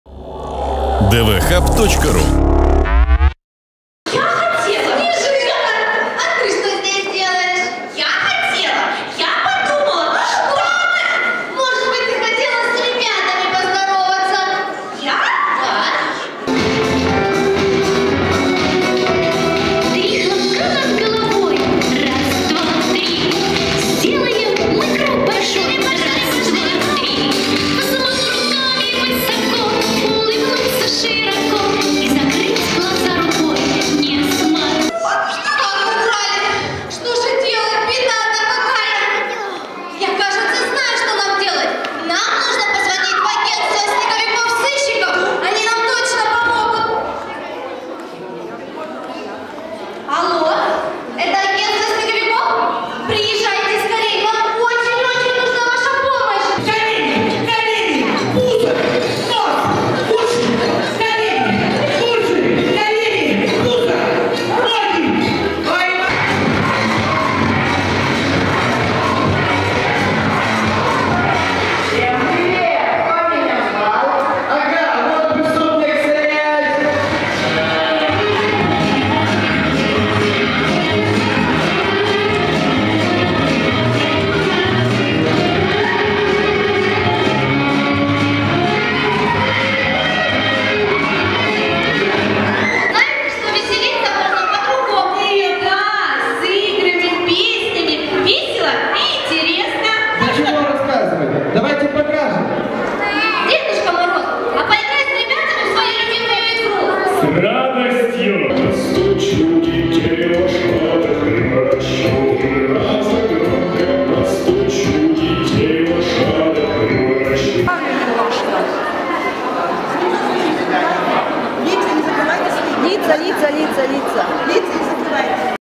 Новогоднее представление в Биробиджане для детей с ограниченными физическими возможностями
В биробиджанском дворце культуры состоялось новогоднее представление для детей с ограниченными физическими возможностями. Организатором яркого праздника, собравшего сто участников, выступил комитет социальной защиты правительства ЕАО. Подробности